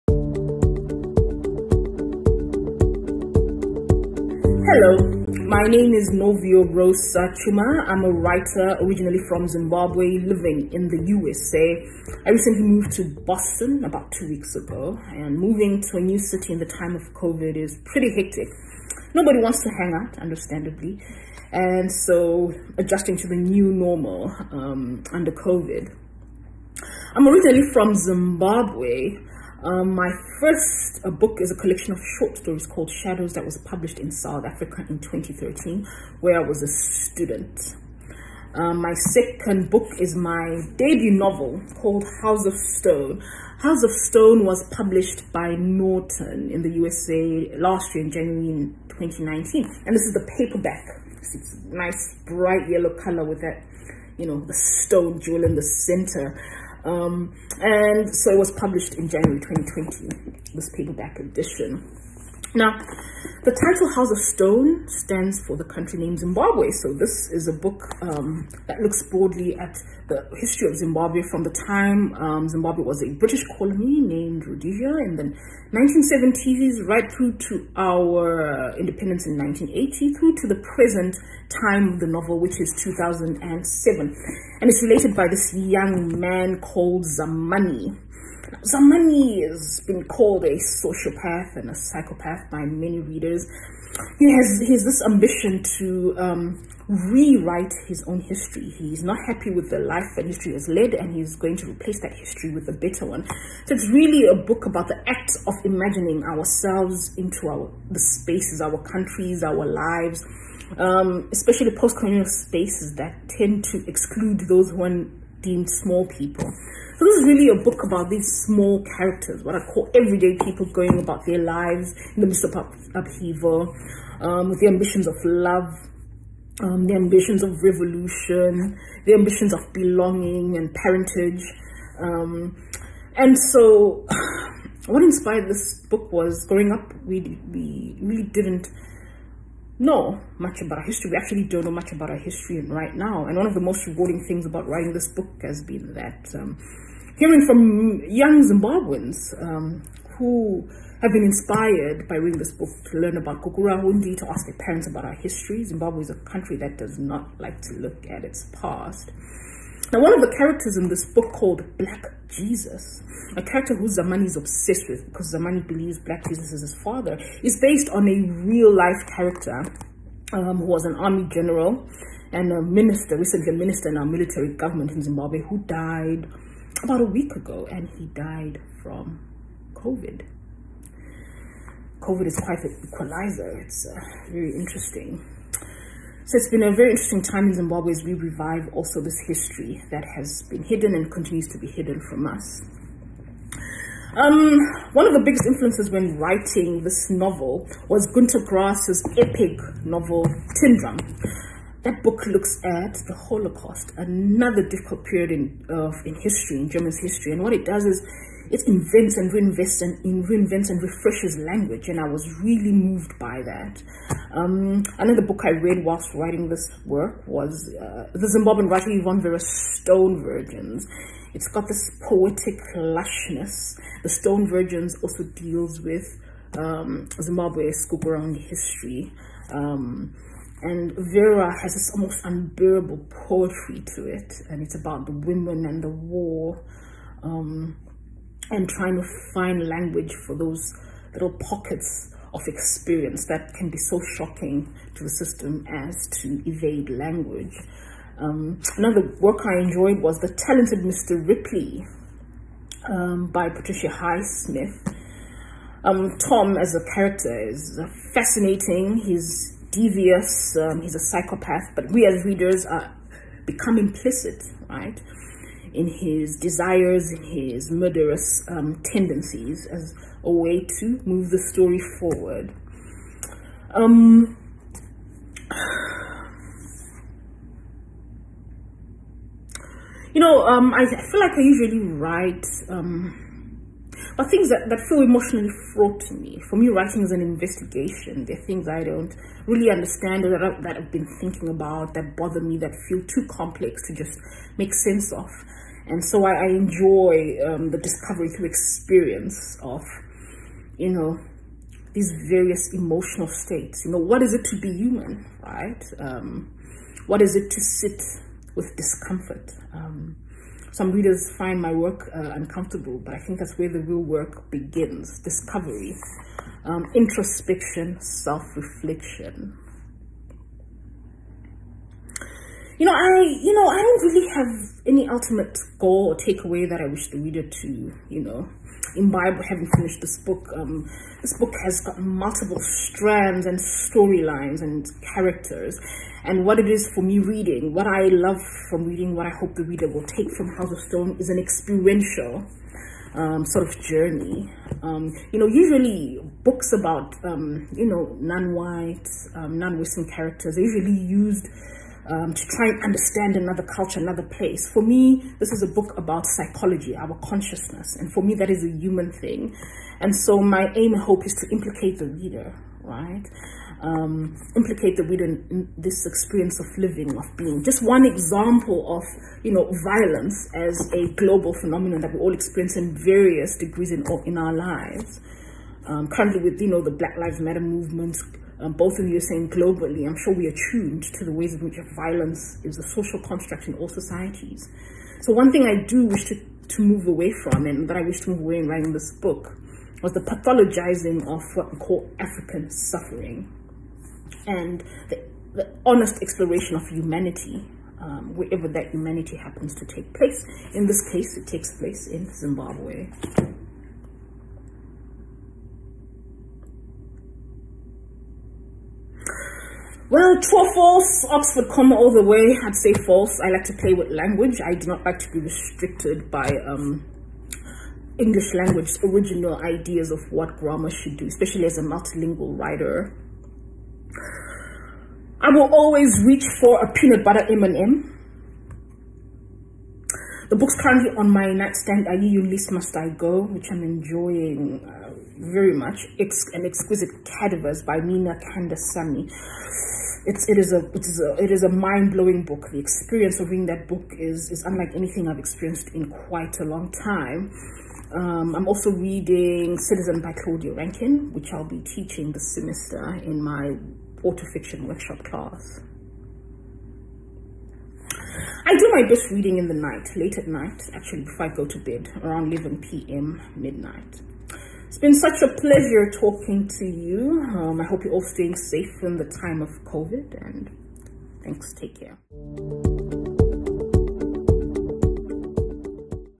Novuyo Rosa Tshuma, Harbor Springs Festival of the Book, Meet the Presenters 2020
Meet Novuyo Rose Tshuma!